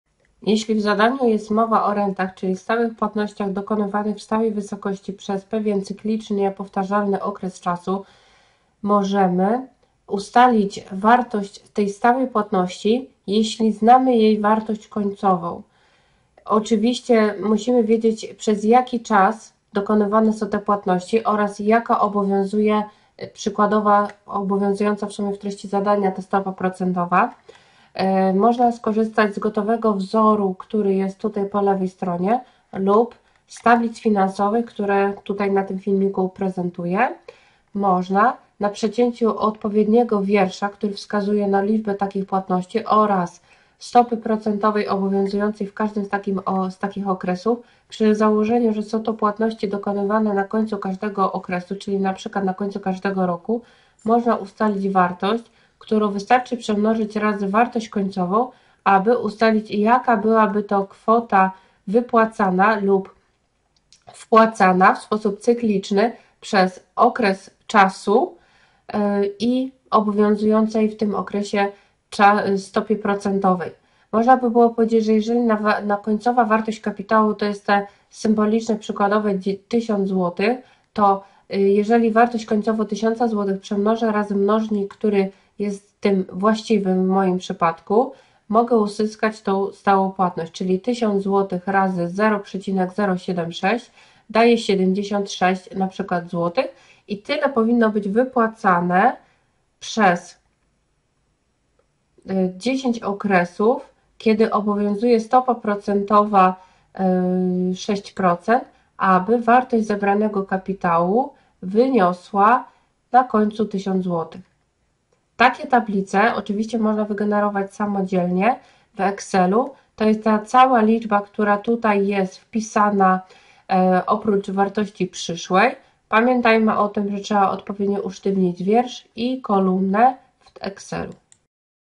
Tablice finansowe wartość renty wg wartości przyszłej rent z dołu mnożnik kalkulator - korzystanie
Wszystkie tablice finansowe w 1 pliku PDF dostępne są na stronie
Linki do wszystkich filmów:

Słowniczek:
Odsetki proste naliczane są od stałej podstawy z okresu początkowego, a odsetki złożone - od podstawy kapitału zwiększanej o dotychczasowe odsetki.
PV - (ang. Present Value) - bieżąca (obecna) wartość kapitału np. lokaty/kredytu
FV - (ang. Future Value) - przyszła (końcowa) wartość kapitału np. lokaty/kredytu
FVA - (ang. Future Value Annuitet) - przyszła (końcowa) wartość rent
PVA - (ang. Present Value Annuitet) - bieżąca (obecna) wartość rent
FVA - (ang. Future Value Annuitet) - przyszła wartość rent
"z dołu" - na koniec okresu np. miesiąca/roku - domyślny rodzaj płatności
"z góry" - na początku okresu np. miesiąca/roku - rodzaj płatności wyraźnie określony w treści zadania.

Film jest poświęcony tematyce wartości renty i jak ją obliczyć za pomocą mnożnika i kalkulatora. Omawia przykładowe zadanie ilościowe, które pomaga wyjaśnić proces obliczania wartości renty. Film jest skierowany do osób, które chcą zrozumieć temat finansów i ekonomii.

W filmie zostaną poruszone następujące tematy:

    Tablice finansowe i ich zastosowanie
    Wartość renty wg wartości przyszłej rent
    Zastosowanie mnożnika i kalkulatora w obliczeniu wartości renty
    Korzystanie z tych narzędzi w praktyce

Jeśli jesteś zainteresowany/a tematyką finansów i ekonomii, to ten film jest dla Ciebie. Serdecznie zachęcam do obejrzenia i mam nadzieję, że pomoże on w zrozumieniu tej dziedziny.